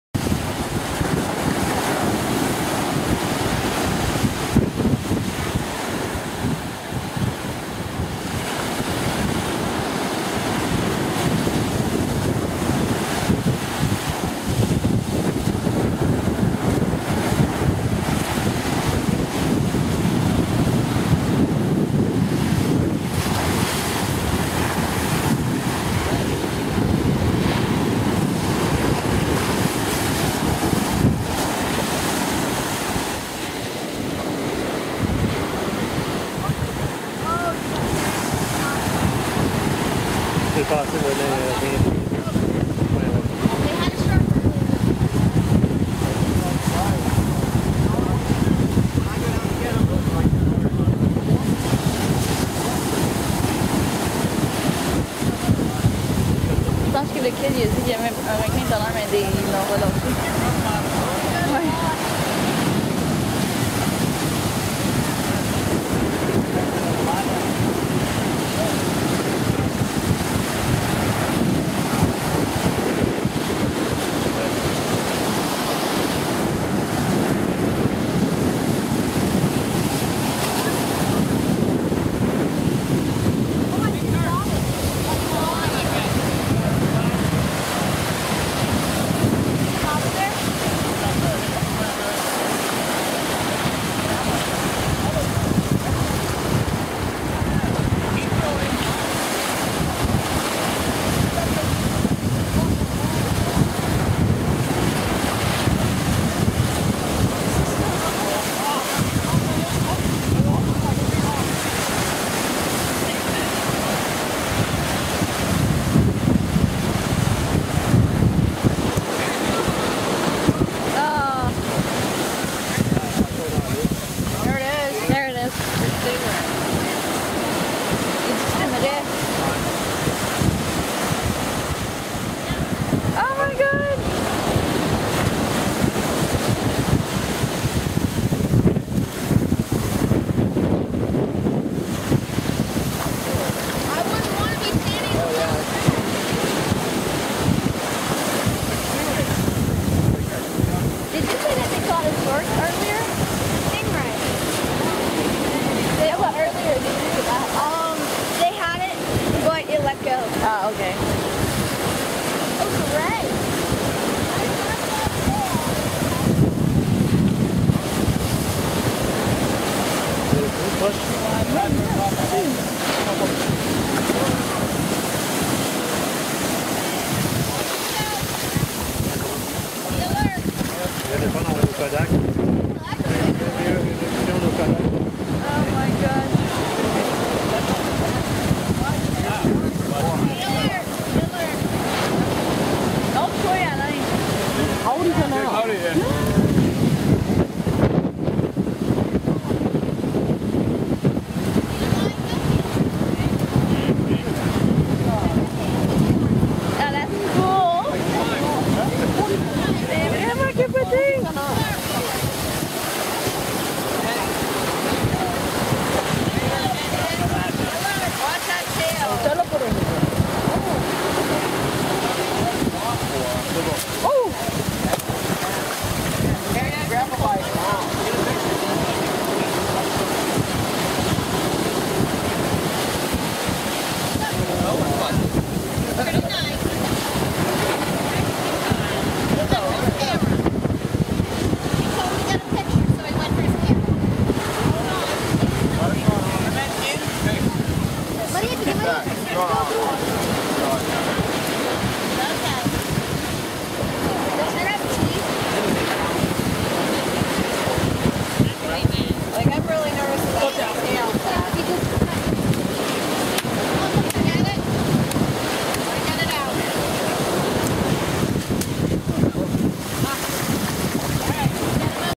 Amazing,beautiful stingray at Assateague Island
Stingray at Assateague island August 2013
AMAZING first time seeing a stingray.